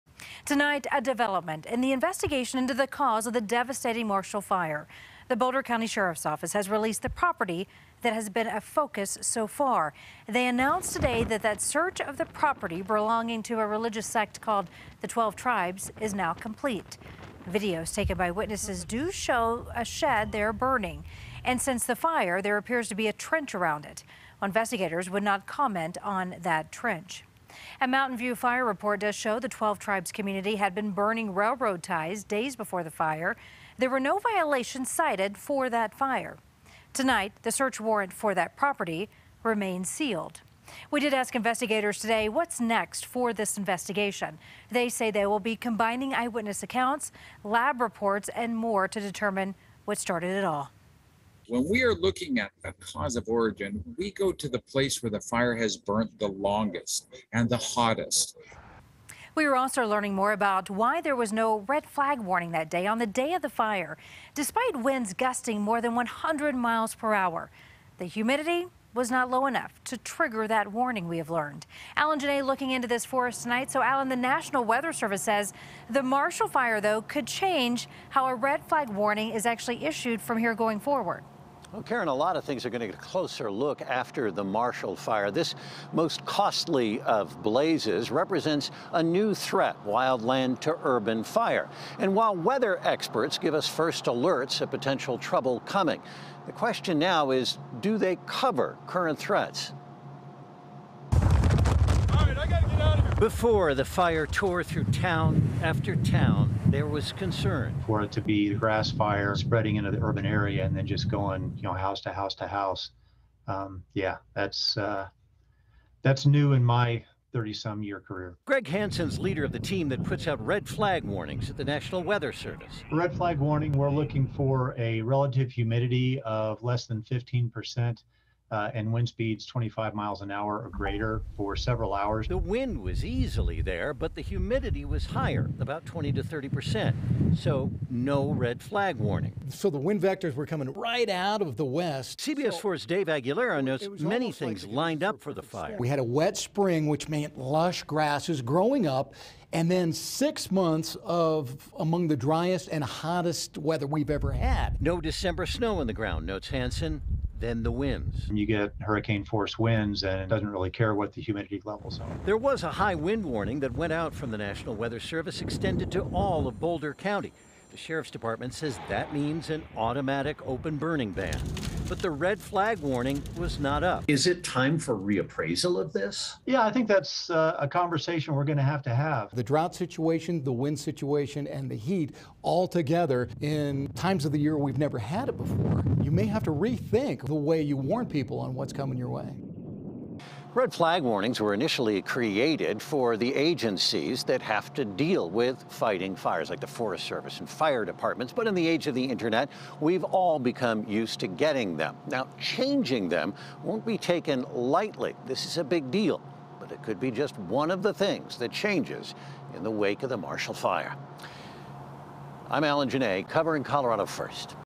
Experts Explain Why No Red Flag Warning Issued Before Marshall Fire
Wind speeds greater than 100 mph are not unheard of, but it was the combination that was different.